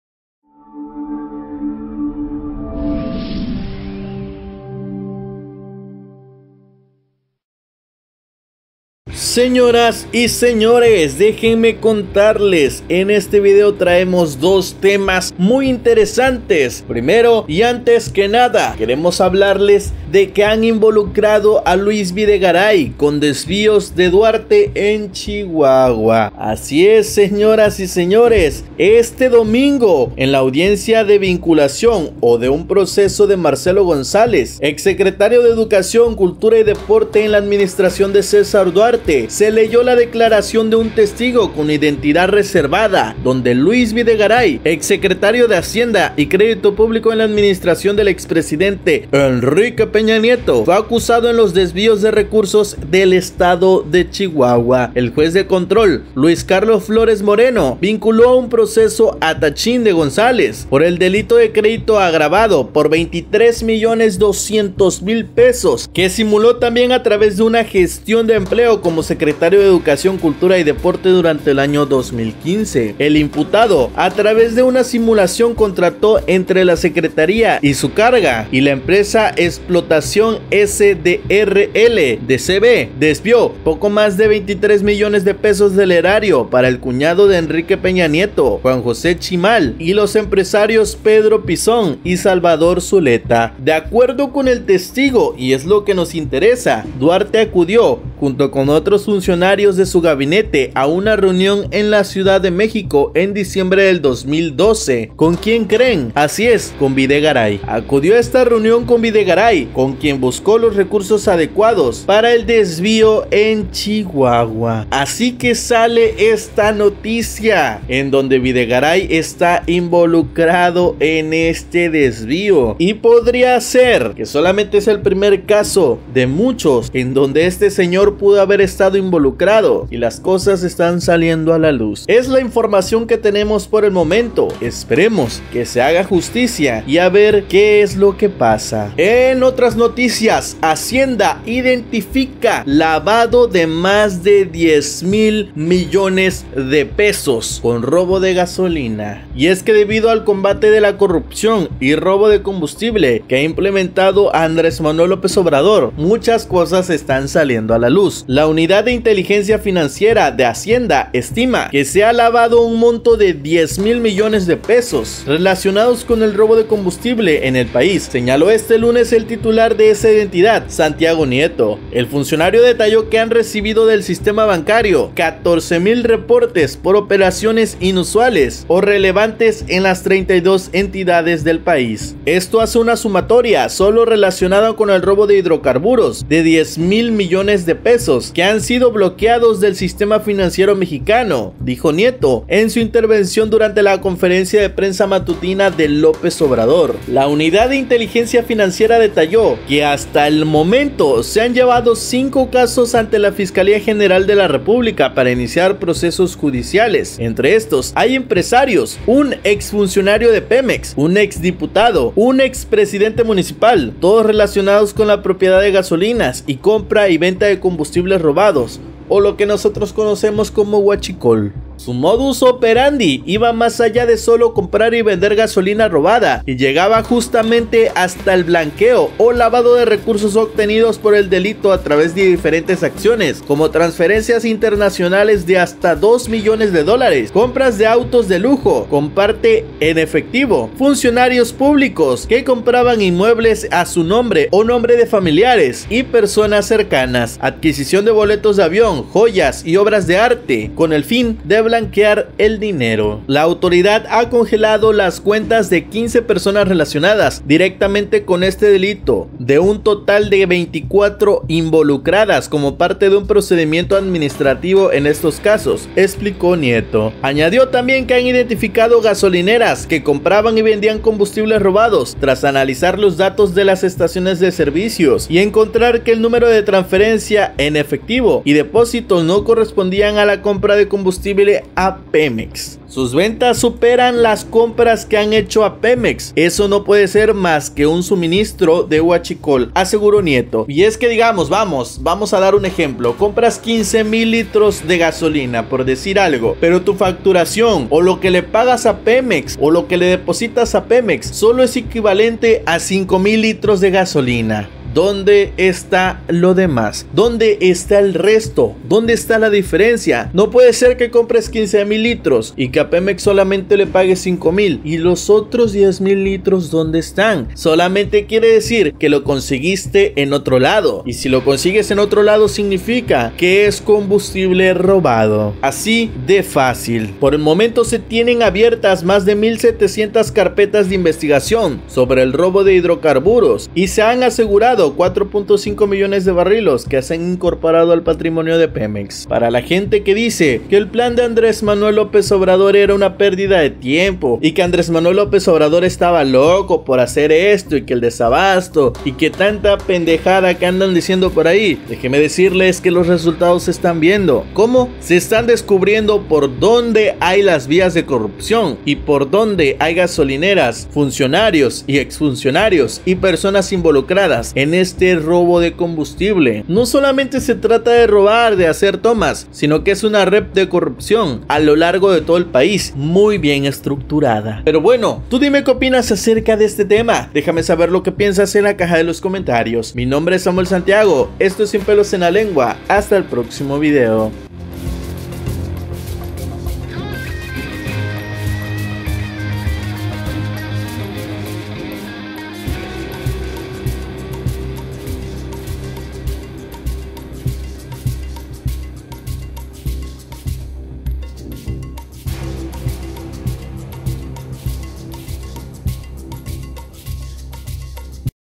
VIDEGARAY involucrado en DESVIÓ MILLONARIO|Hacienda descubre LAVADO DE DINERO en ROBO DE GASOLINA ⛽🔥
°°°°°NOTICIA°°°°°

 La Unidad de Inteligencia Financiera (UIF) de Hacienda estima que se ha lavado un monto de 10,000 millones de pesos (mdp) relacionados con el robo de combustibles en el país, señaló este lunes el titular de esa entidad, Santiago Nieto.

El funcionario detalló que han recibido del sistema bancario 14,000 reportes por operaciones inusuales o relevantes en las 32 entidades del país.

"Esto hace una sumatoria solo relacionada con el robo de hidrocarburos de 10,000 mdp que han sido blanqueados al sistema financiero mexicano”, dijo Nieto en su intervención durante la conferencia de prensa matutina de López Obrador.

EDICION POR: SAMUEL SANTIAGO / MAHATMA RIVERO/DAYANNA GUTIERREZ
EDICION DE VIDEO: MAHATMA RIVERO, AARON VERA, SAMUEL SANTIAGO.

­ ­ ◥◣◥◣◥◣COPYRIGHT◥◣◥◣◥◣
Lα cσpiα dε εstε vidεσ,
sεяα dεnunciαdα.
ORIGINAL